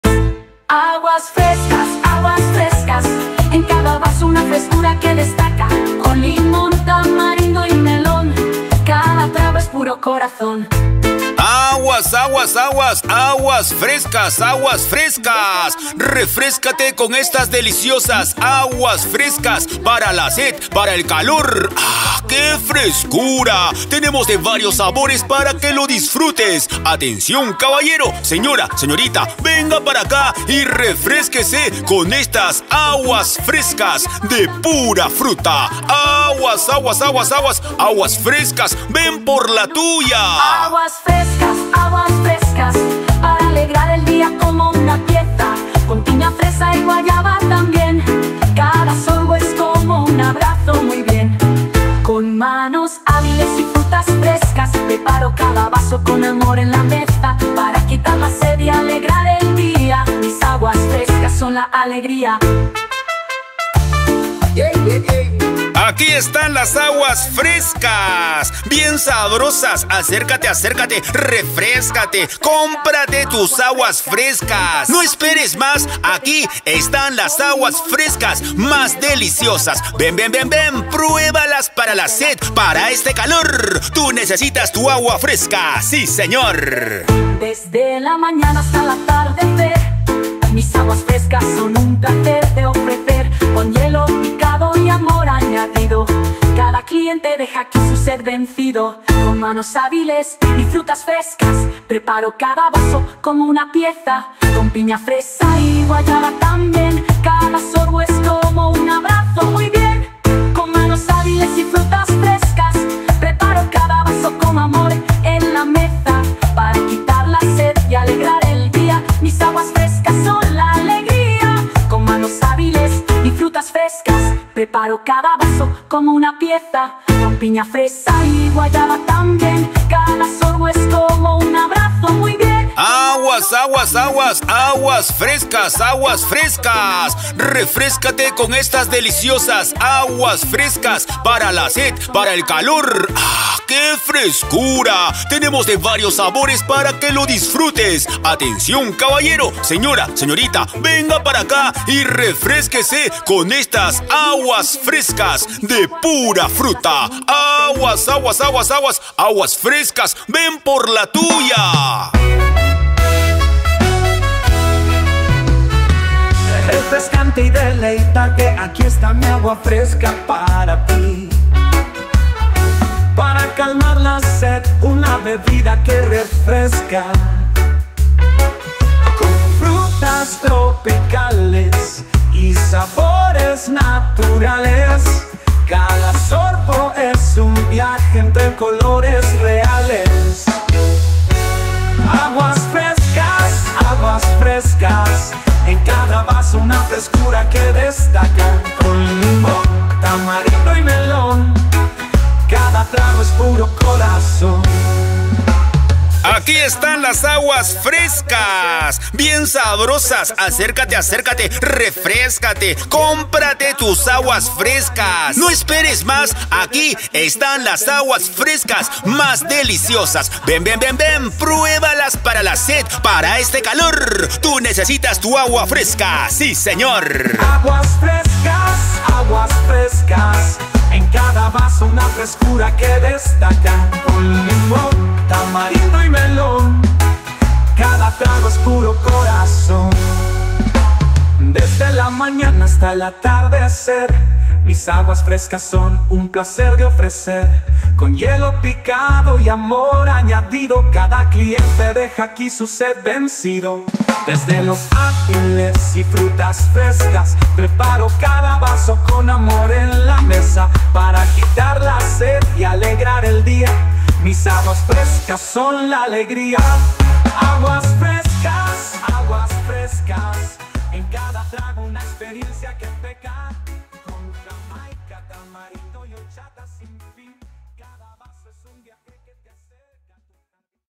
📣VOZ y MUSICA para vender AGUAS FRESCAS
ya estamos en Spotify.
canciones y pregones por el día del padre